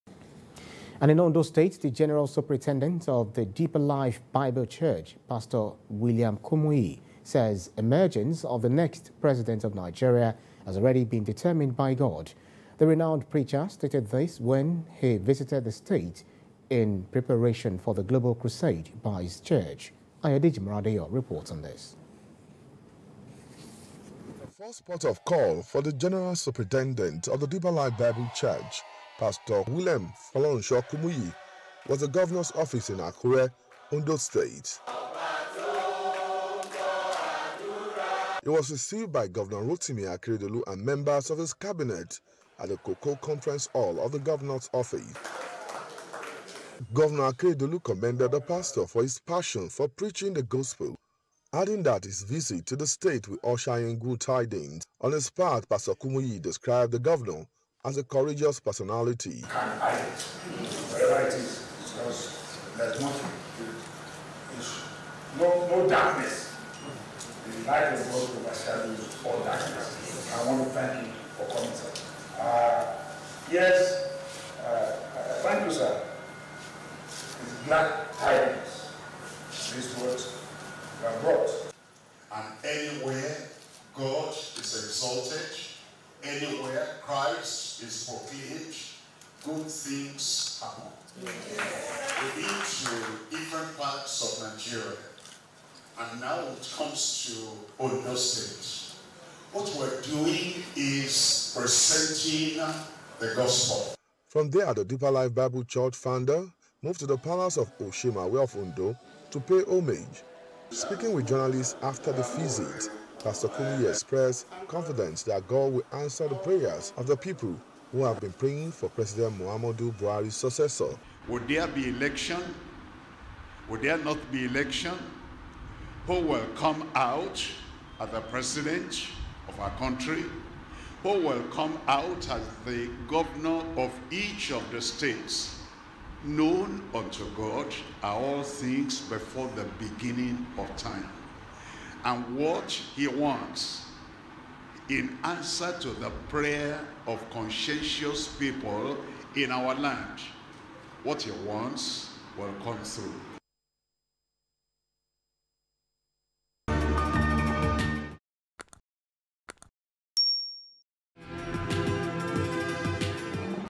WATCH: Pastor Kumuyi Says God Has Determined Nigeria's Next President
The General Superintendent of the Deeper Life Bible Church, Pastor William Kumuyi, says the emergence of the next President of Nigeria, has already been determined by God.